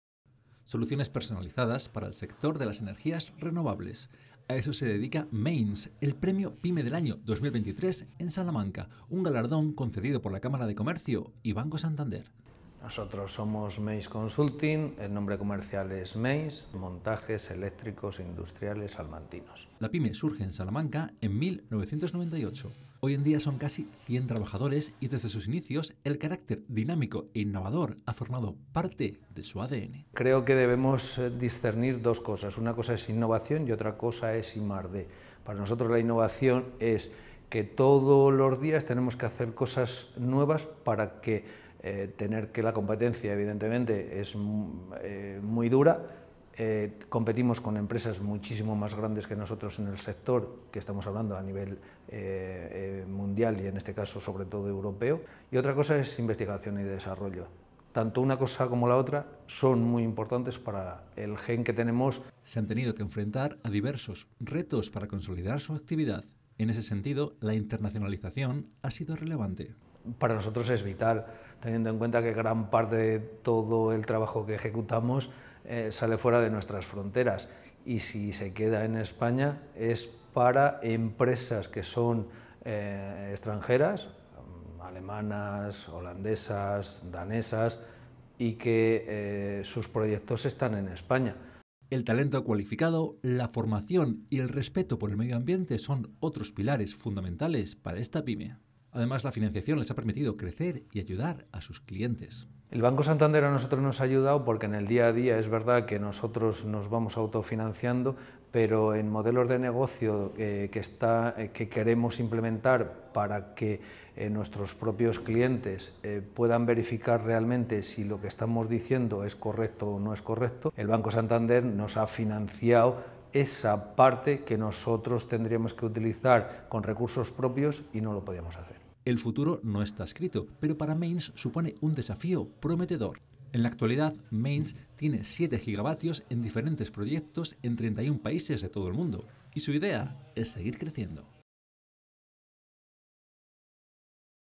Meins, especialista en soluciones para renovables, premio Pyme del Año en Salamanca
Soluciones personalizadas para el sector de las energías renovables.

A eso se dedica Meins el premio Pyme del Año 2023 en Salamanca, un galardón concedido por la Cámara de Comercio y Banco Santander.

La pyme surge en Salamanca en 1998. Hoy en día son casi 100 trabajadores y desde sus inicios el carácter dinámico e innovador ha formado parte de su ADN.

Se han tenido que enfrentar a diversos retos para consolidar su actividad. En ese sentido la internacionalización ha sido relevante.

El talento cualificado, la formación y el respeto por el medioambiente son otros pilares fundamentales para esta pyme.
Además la financiación les ha permitido crecer y ayudar a sus clientes.

El futuro no está escrito, pero para Meins supone un desafío prometedor. En la actualidad Meins juega con 7 GW en diferentes proyectos en 31 países de todo el mundo. Y su idea es seguir creciendo.